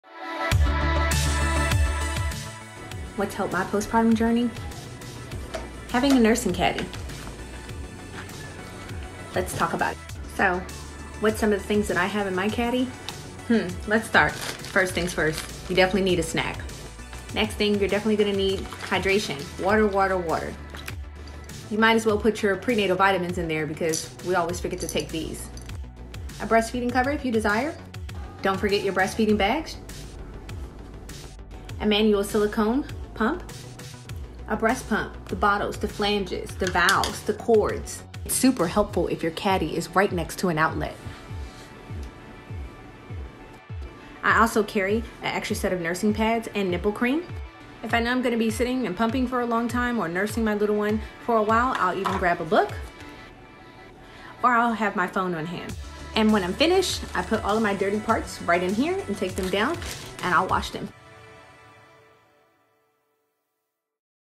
What's In My Nursing Caddy
A breastfeeding basket is essentially a care package of nursing necessities kept together in a caddy or container. You collect everything you would typically need for a nursing session and put them together in a caddy. It saves you a lot of time from hunting for items you need every time you breastfeed.